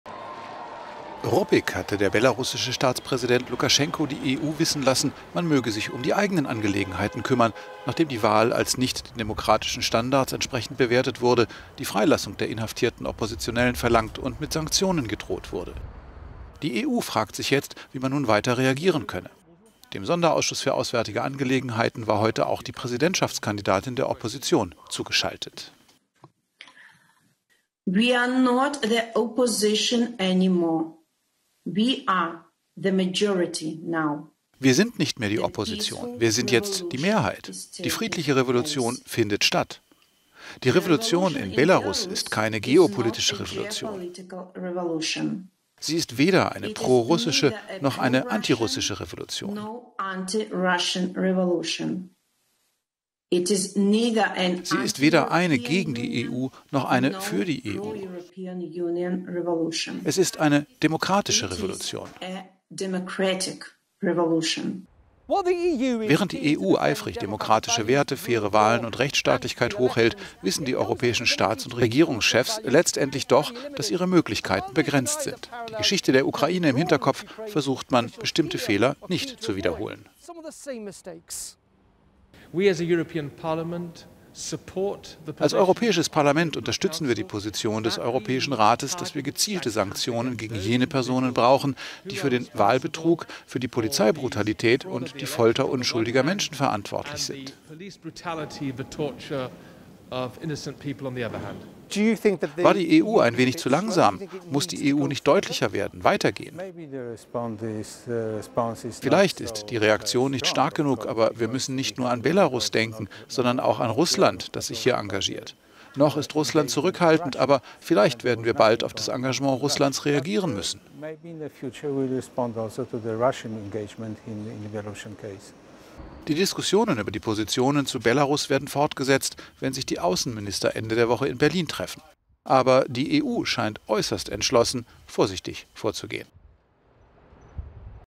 EU debattiert Lage in Belarus
"Wir sind nicht mehr die Opposition, wir sind jetzt die Mehrheit." so die live zugeschaltete Swetlana Tichanowskaja, belarussische Oppositionsführerin im litauischen Exil. Die Diskussionen über die Position zu Belarus werden fortgesetzt, wenn sich die Außenminister Ende der Woche in Berlin treffen.